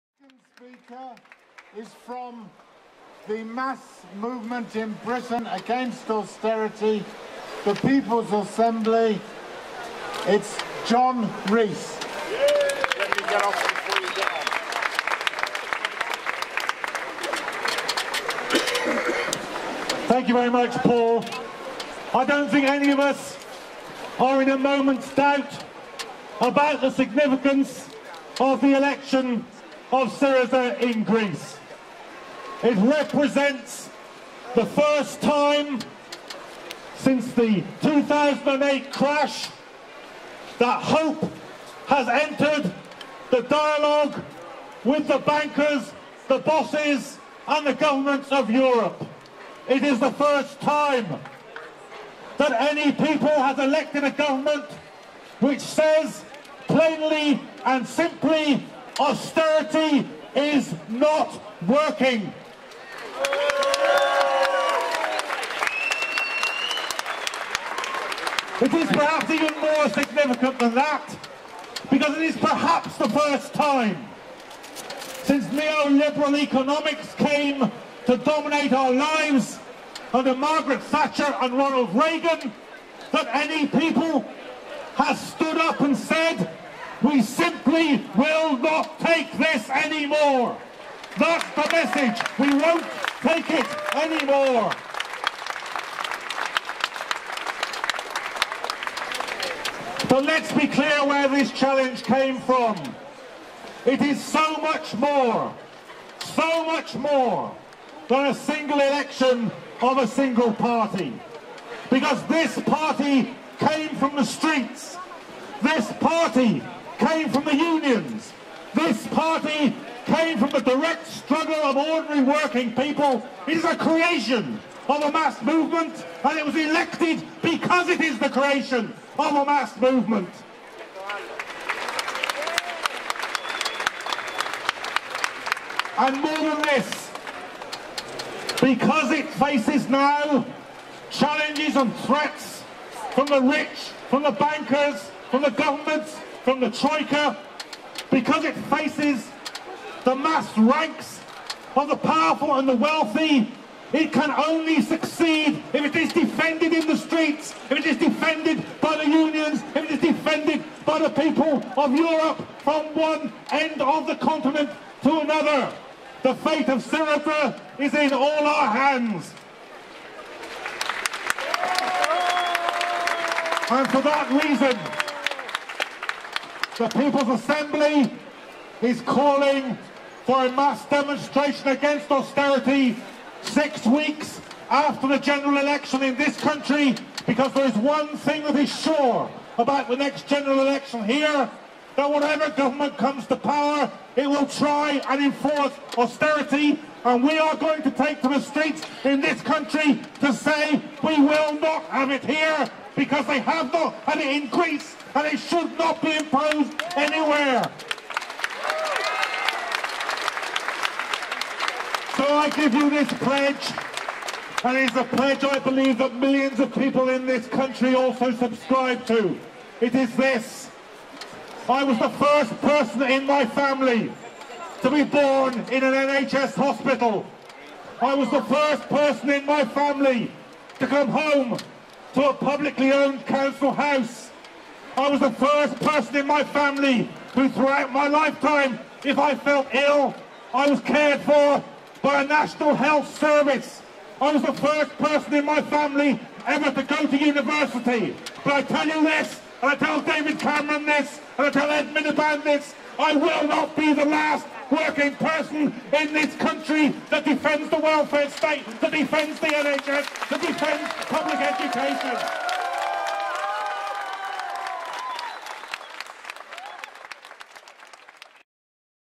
John Rees fom the People's Assembly addresses the Let Greece Breathe rally, Trafalgar Square.
John Rees from the People's Assembly adresses the Let Greece Breathe rally, London